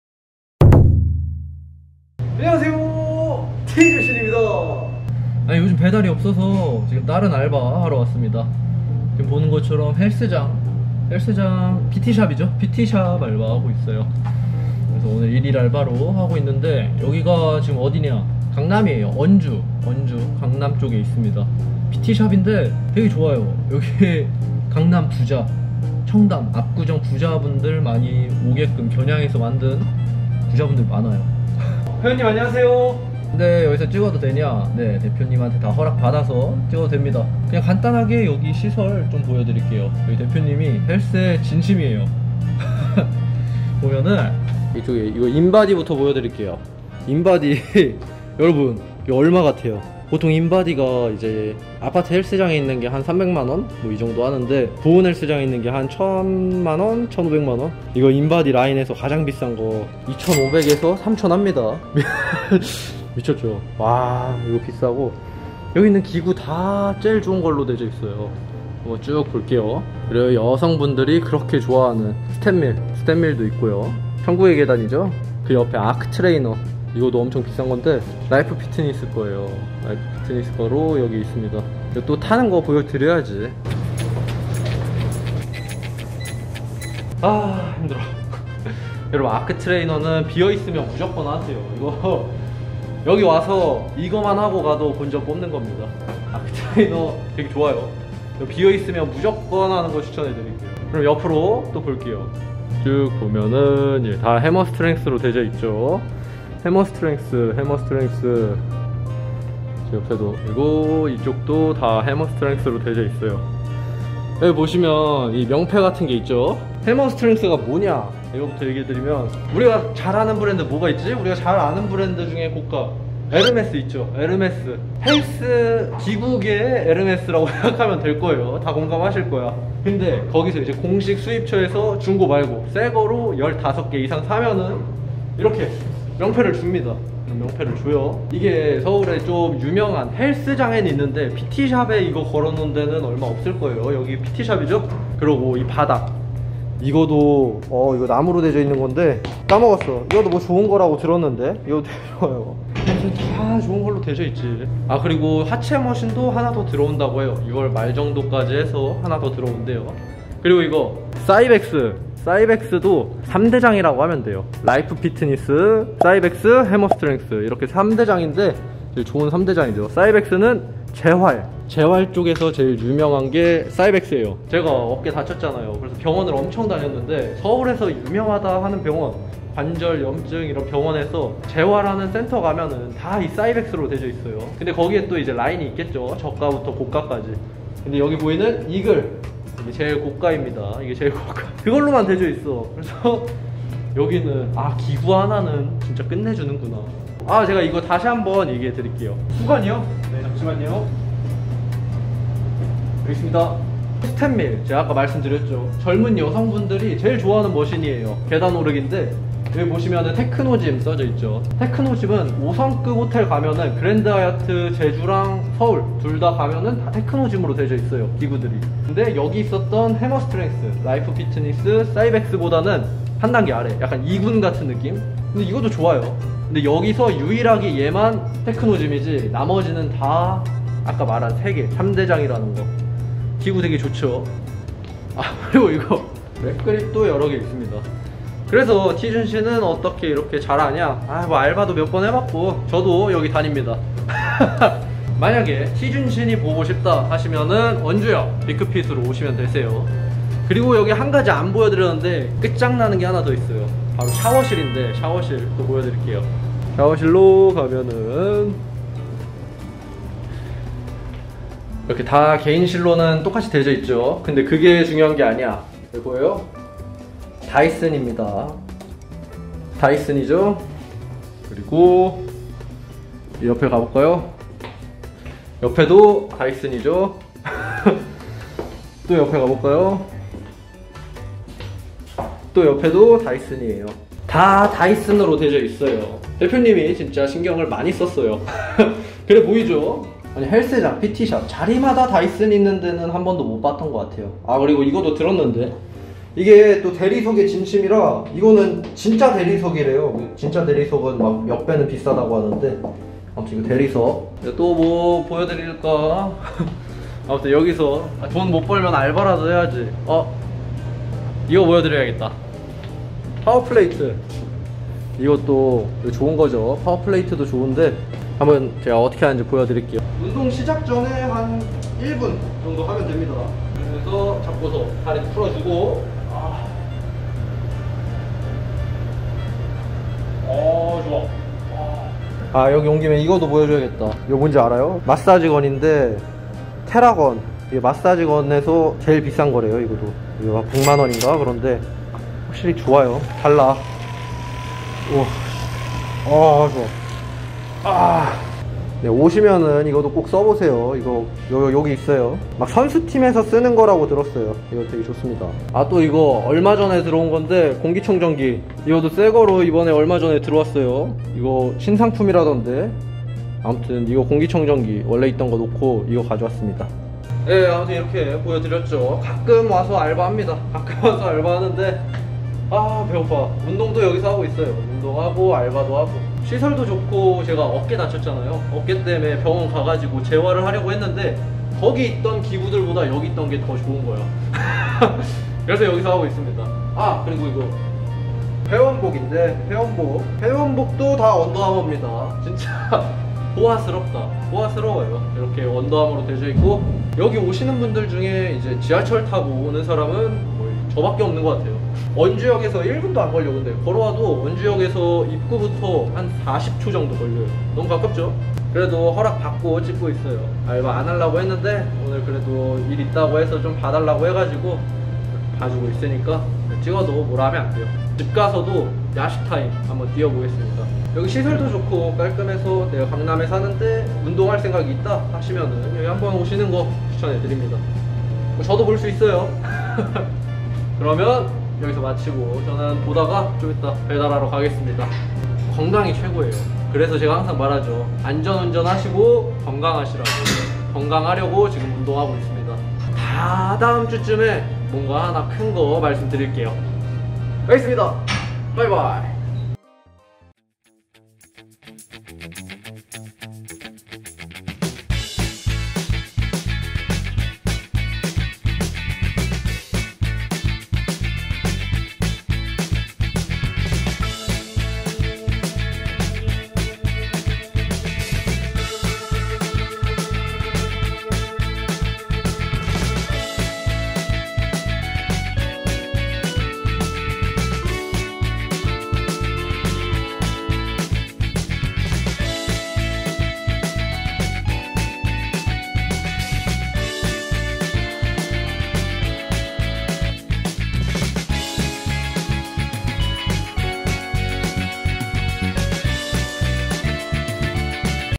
인바디만 3천만원?! 강남 금수저 헬스장 일일 알바하기. 배달이 없으니 알바라도!! |헬스장브이로그|헬스|헬창|해머스트랭스|피크핏|헬린이
배달이 없죠.. 금요일에 점심배달하고 알바하러 넘어온겁니다
기구가 워낙 좋아서 어깨치료하면서 재활에 많이 도움됐어요

운동하실분들은 추천합니다!

위치 : 서울특별시 강남구 봉은사로 211 그림바우빌딩 지하1층